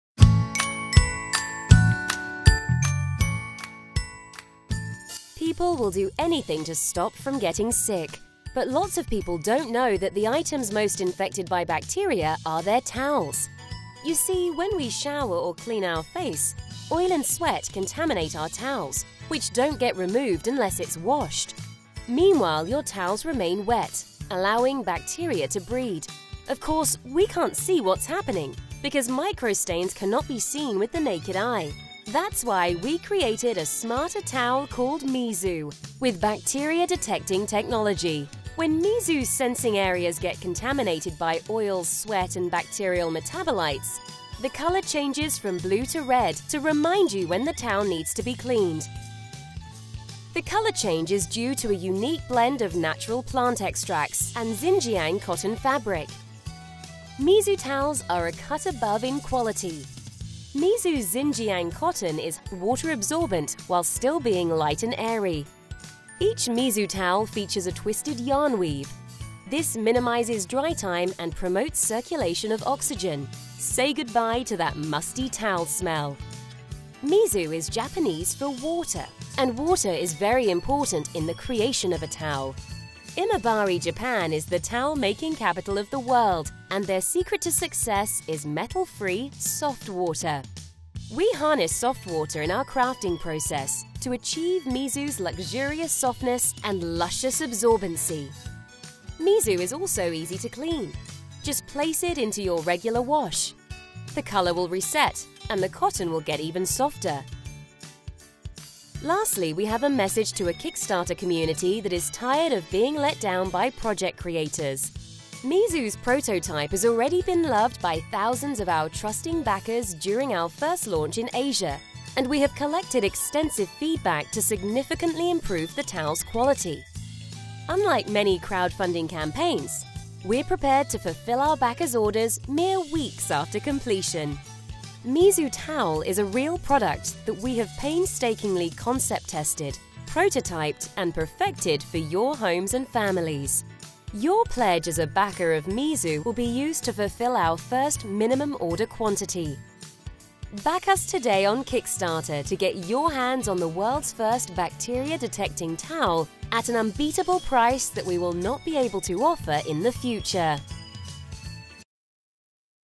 Mizu Smart Towel | The Softest and Most Durable Towel Ever by Team Mizu
Mizu Smart Towel from Kickstarter
A game-changing bacteria sensing smart textile made of premium Xinjiang cotton and world class Japanese craftsmanship in every stitch.